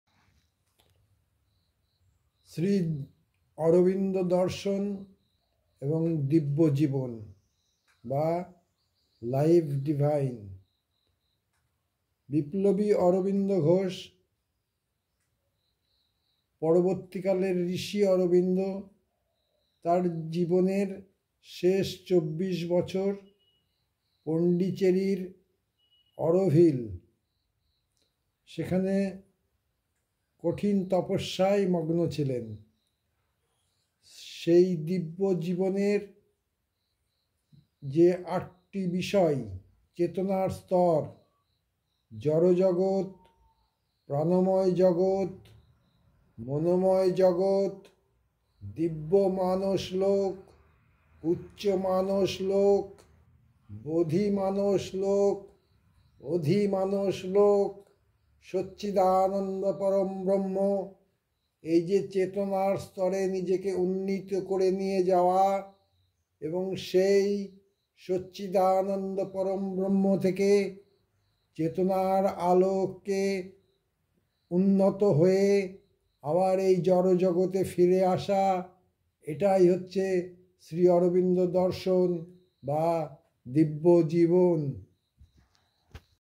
দিব্য-জীবন !! ঋষি শ্রীঅরবিন্দ দর্শন#3see572
বিপ্লবী শ্রী অরবিন্দ ঘোষ থেকে ঋষি শ্রীঅরবিন্দের রূপান্তর কিন্তু আরো বৃহৎ বিপ্লব।পণ্ডিচেরী এবং অরো-ভিল, এখন প্রায় সমার্থক। শ্রীঅরবিন্দের দিব্য-জীবন, উচ্চতম মার্গের গবেষণার বিষয়।
3see572 YouTube channel@3see572
★Sri Arobindo philosophy om anandamayee chaitanyamayee satyamayee parome
★the life divine, "In all things may I see the divine. " Sri Arobindo.